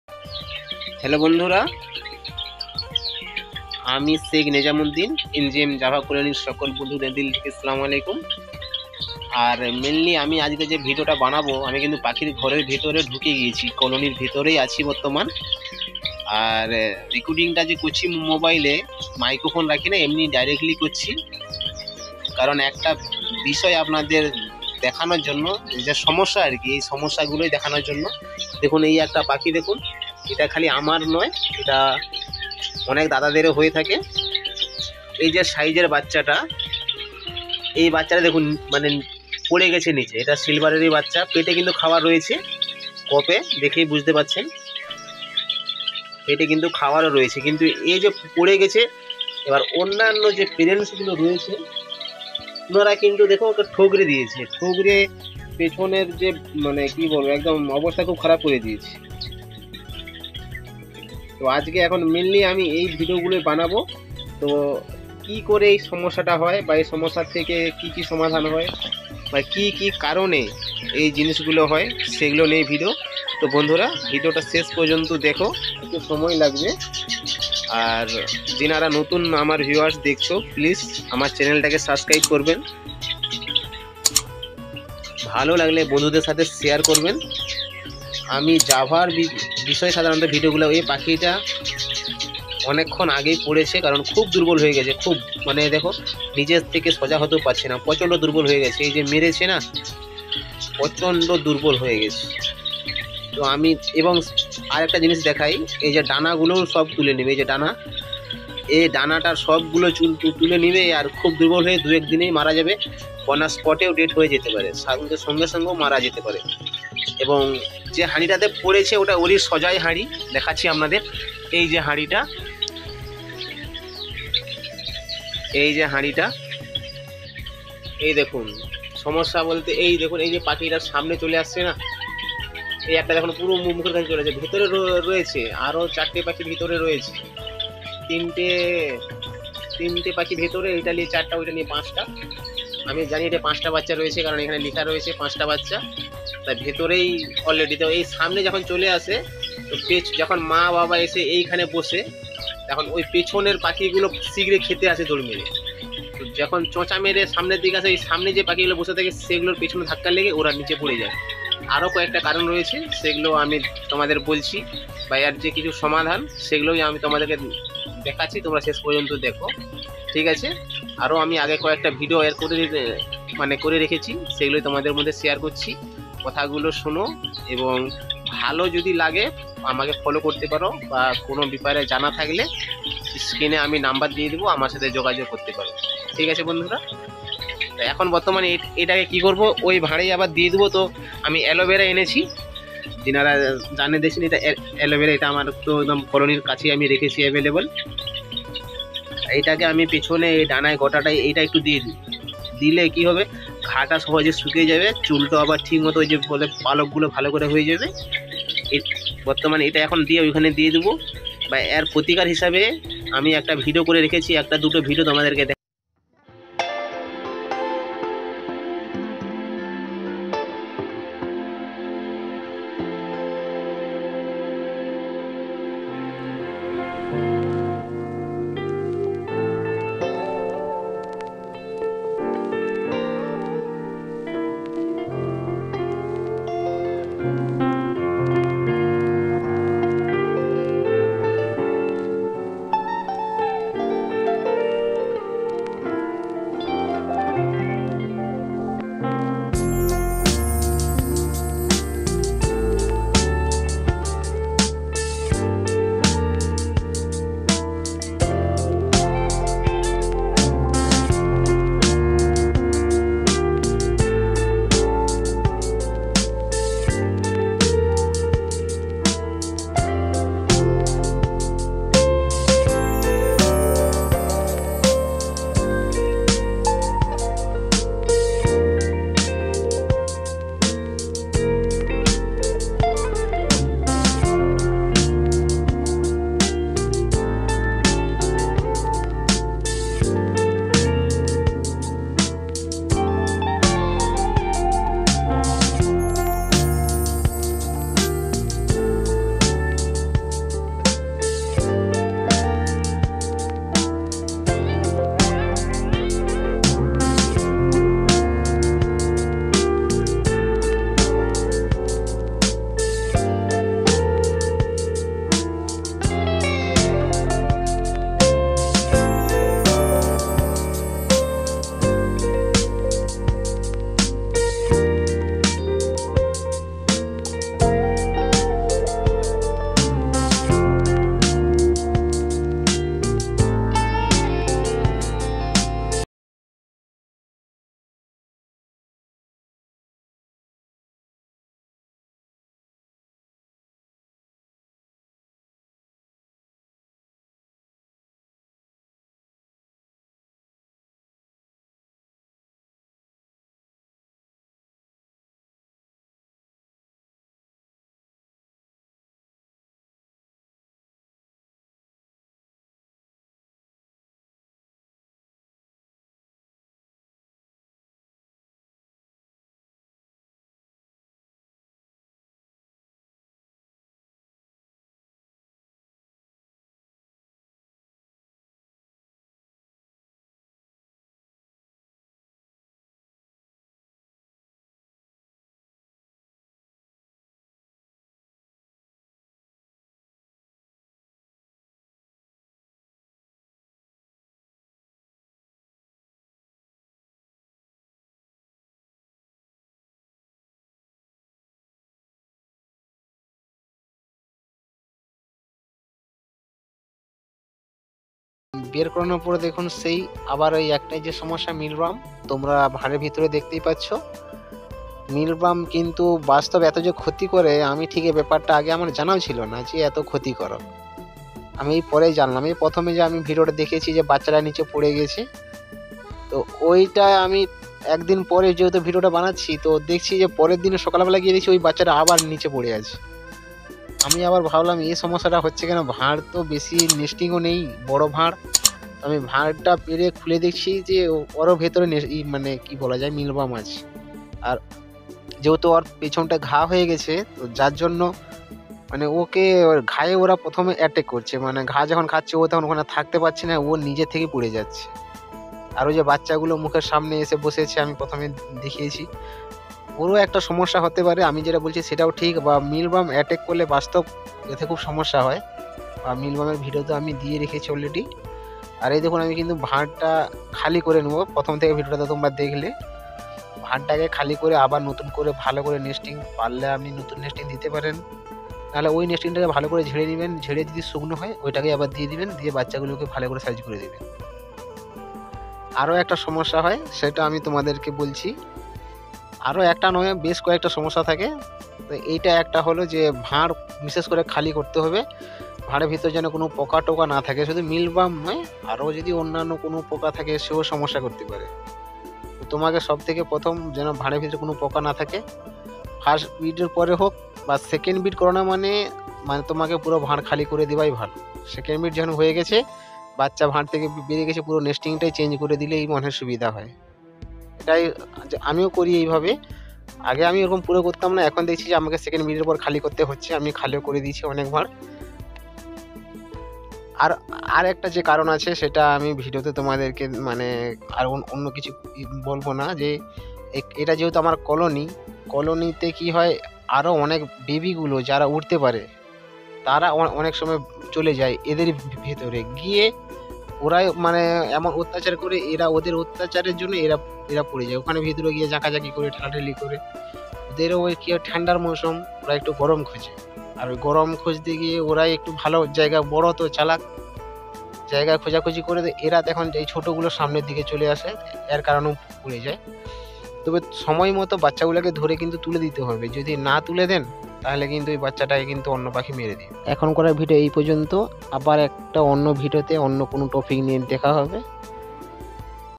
java পাখির ছোট বাচ্চা হাড়ি থেকে পড়ে মারা যাচ্ছে,,কি কারন,আপনি কি করবেন,@Njm505
java পাখির ছোট বাচ্চা হাড়ি থেকে পড়ে মারা যাচ্ছে,, কি কারন,আপনি কি করবেন,‎@njmjavacolonybider5095

name,SK nejamuddin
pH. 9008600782